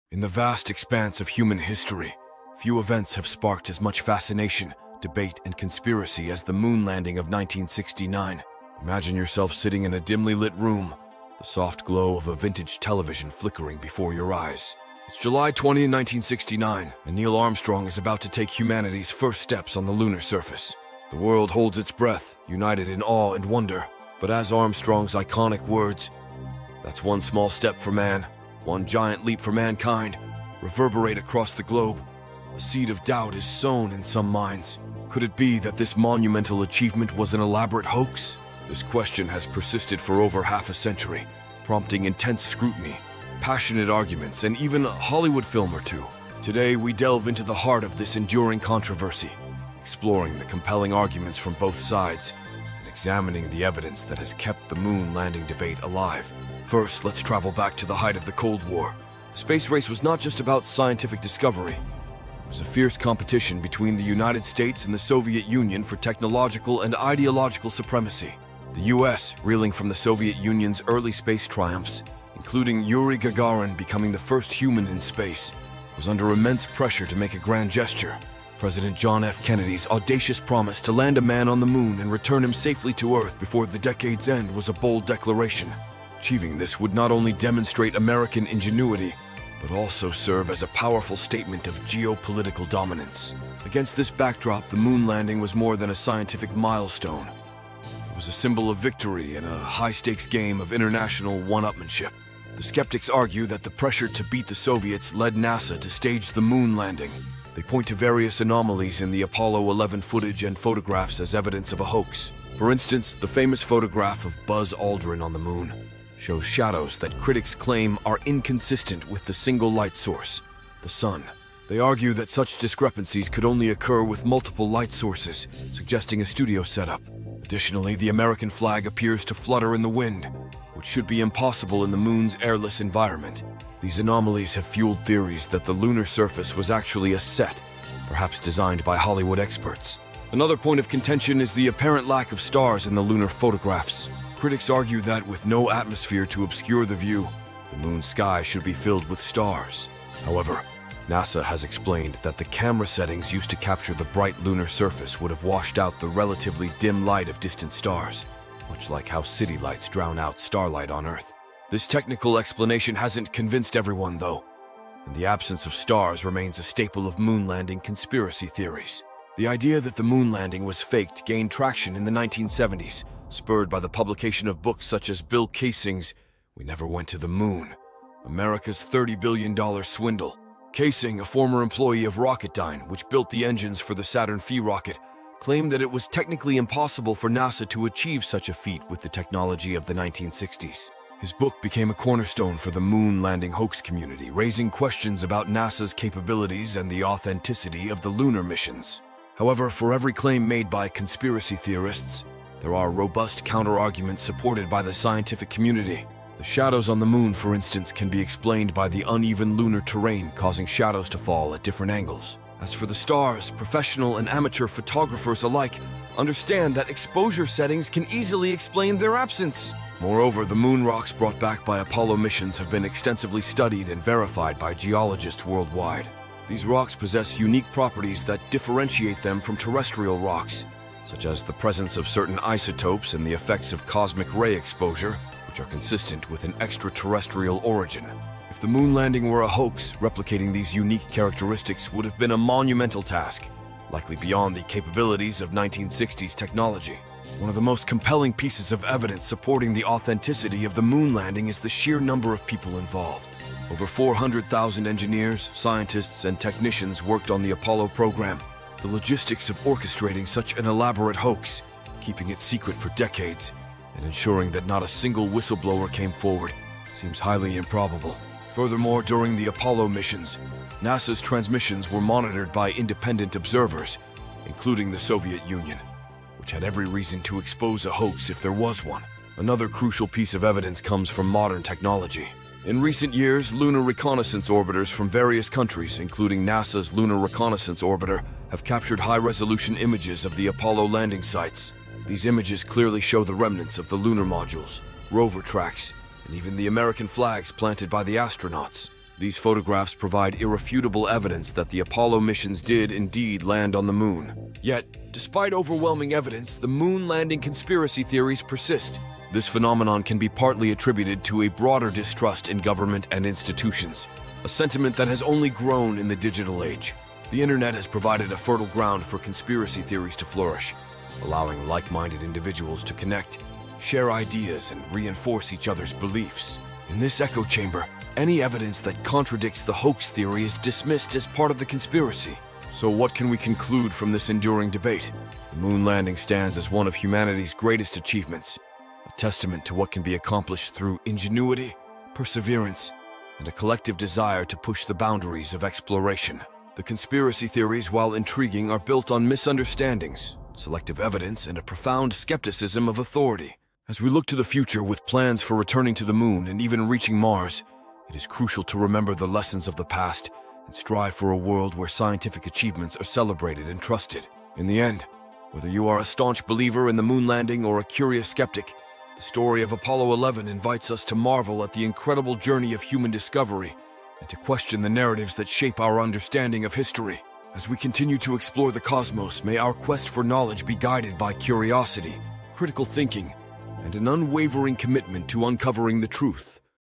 Did We Really Land on the Moon?
In this riveting exploration, we dive deep into one of the most debated events in human history: the 1969 moon landing. Was it the crowning achievement of mankind or an elaborate hoax staged by NASA? Join us as we dissect the arguments from both sides, analyze the anomalies in the Apollo 11 footage, and examine the overwhelming evidence that supports the authenticity of the moon landing. We’ll travel back to the intense atmosphere of the Cold War, unravel the technical explanations behind the alleged inconsistencies, and look at the undeniable proof from modern lunar missions. Whether you’re a firm believer in the moon landing or a curious skeptic, this video promises to challenge your perceptions and ignite your curiosity. Don’t miss this captivating journey into one of the greatest controversies of all time.